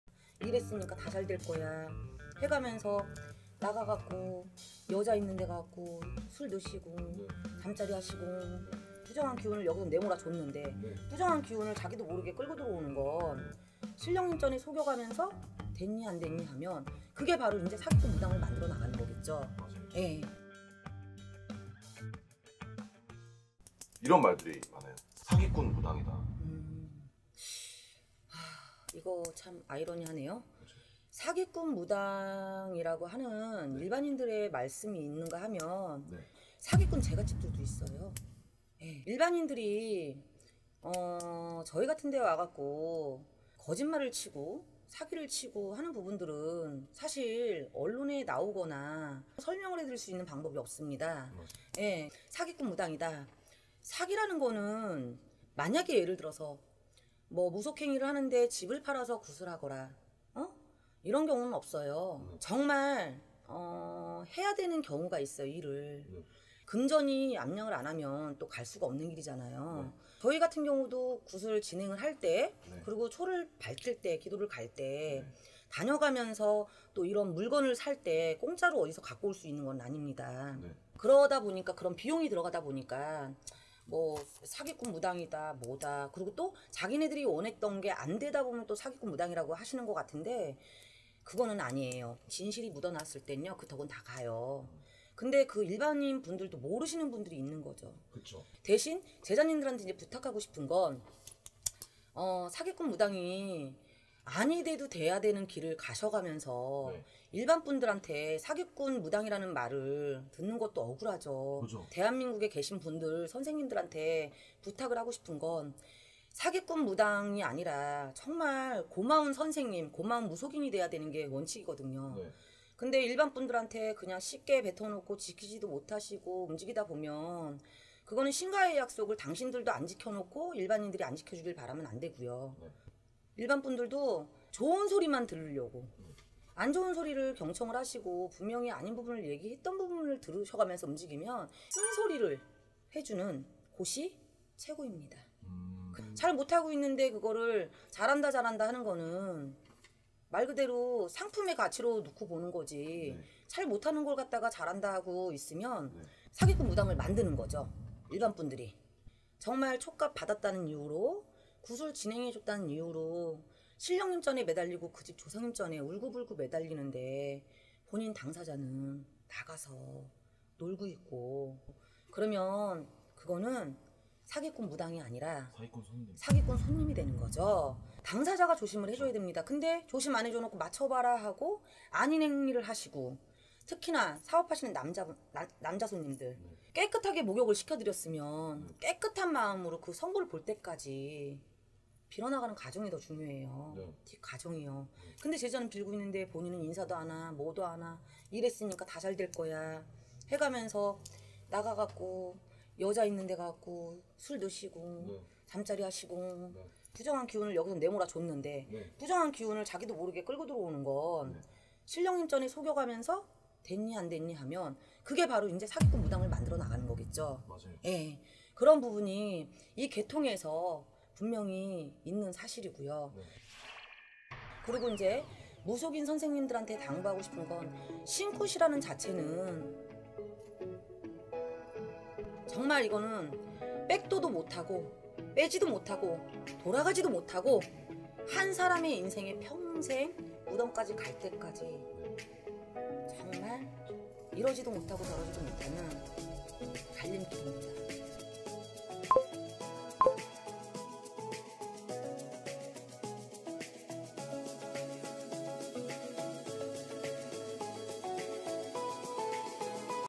"당연한 말에 속지마세요" 무당들의 만행을 고발합니다!! "무당보단 XX도 문제야 문제!" 경기도광주점집 대비마마
자막

★대표블로그★
-올마이티TV